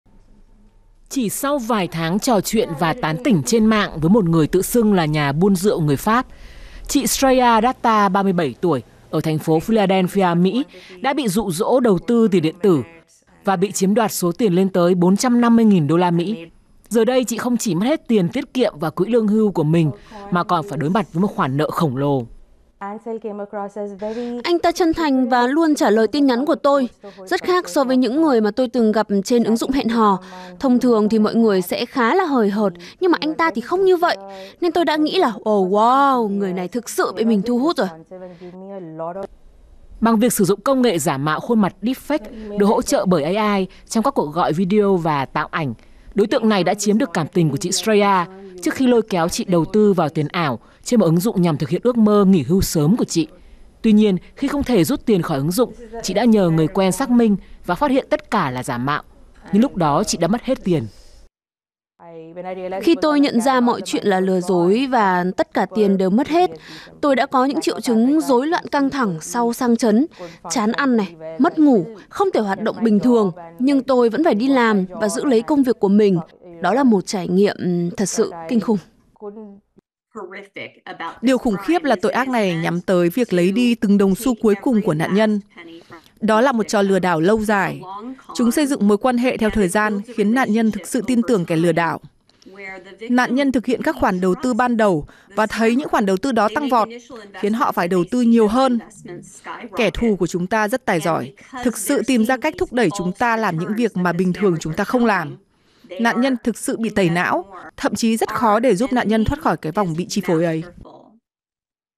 Mỹ: Lừa đảo kiểu "giết thịt lợn" hút hết tiền của nạn nhân | VTV24
Ngỡ là yêu nhưng đến một ngày rồi vỡ tan.

Không chỉ bị lừa mất sạch tiền, những nạn nhân của kiểu lừa "giết thịt lợn" còn bị tổn thương về cả tinh thần.